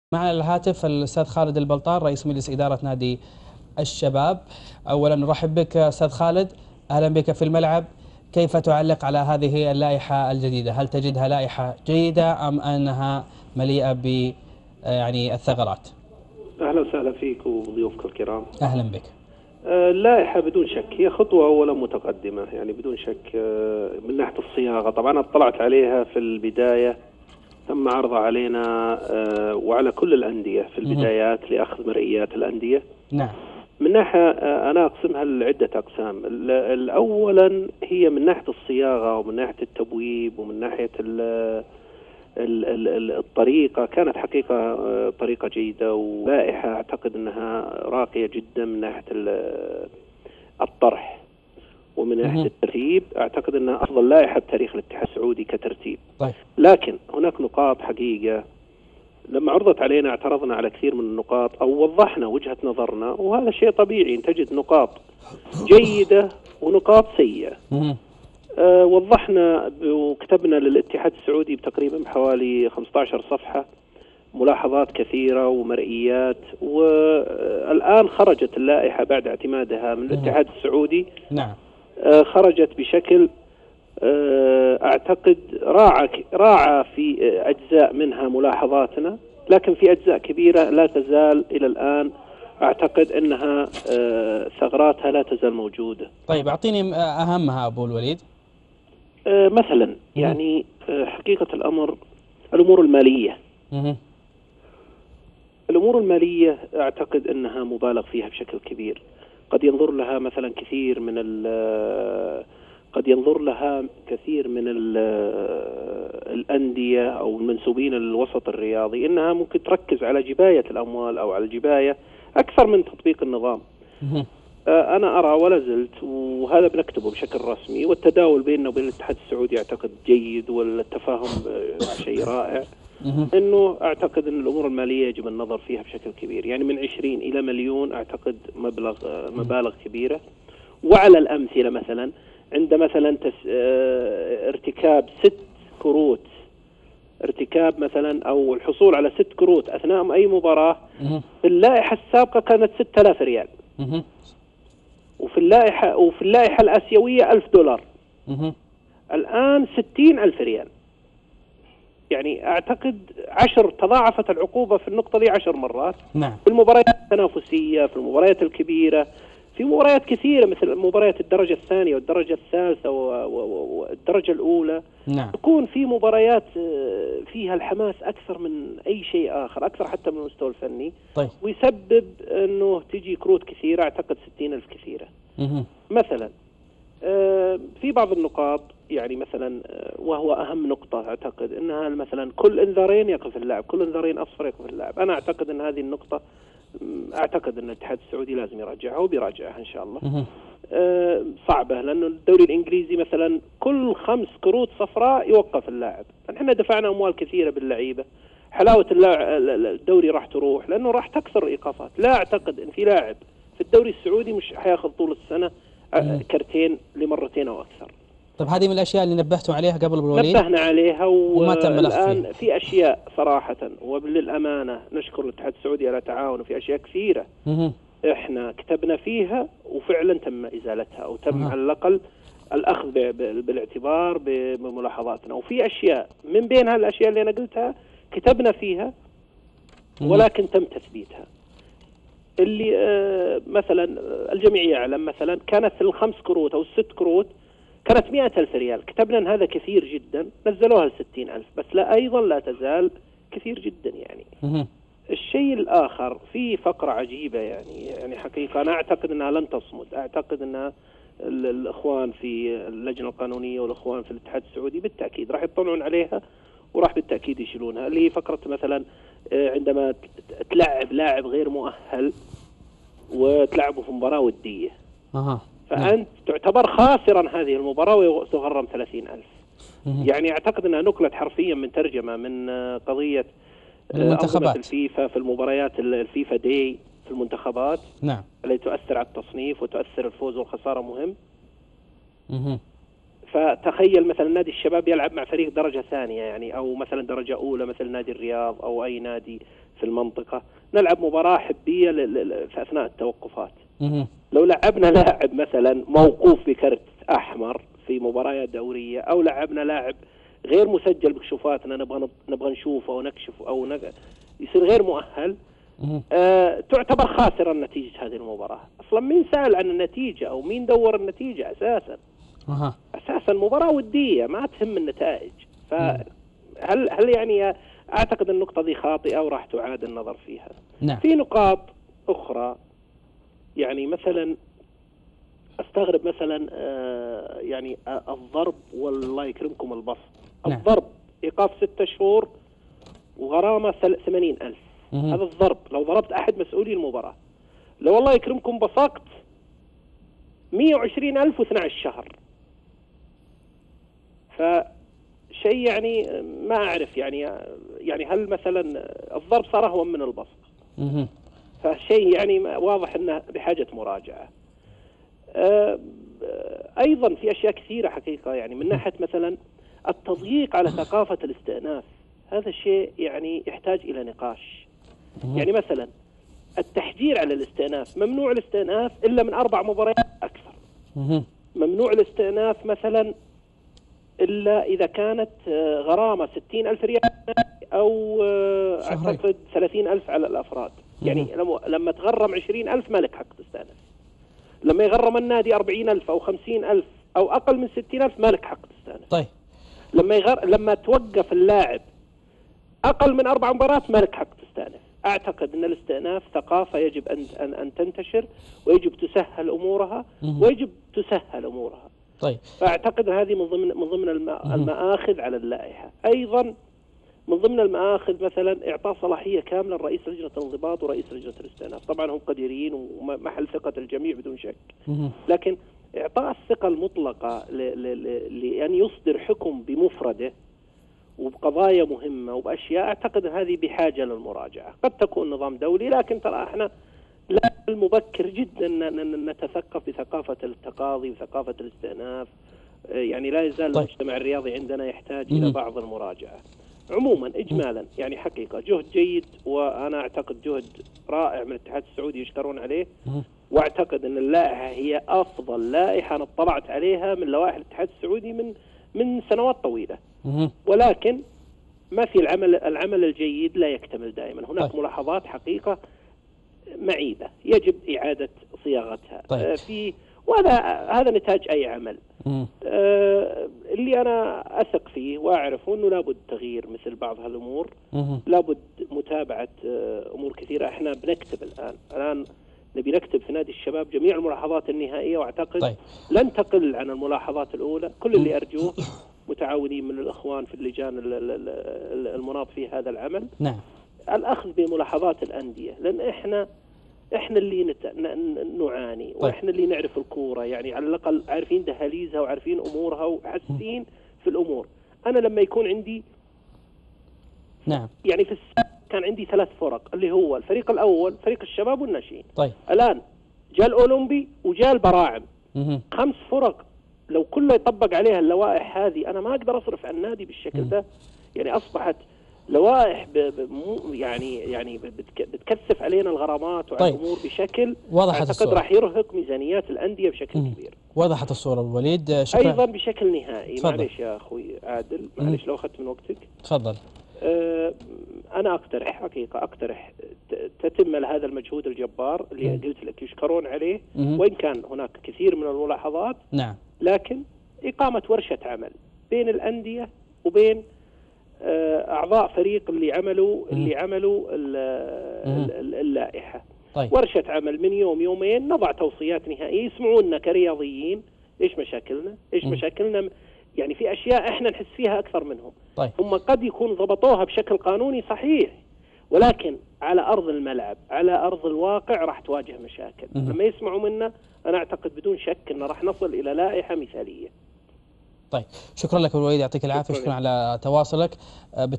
تعليق رئيس نادي الشباب خالد البلطان على لائحة الإنضباط
تعليق رئيس نادي الشباب خالد البلطان على لائحة الإنضباط الجديده
ضيوف الحلقه عادل البطي - سامي القرشي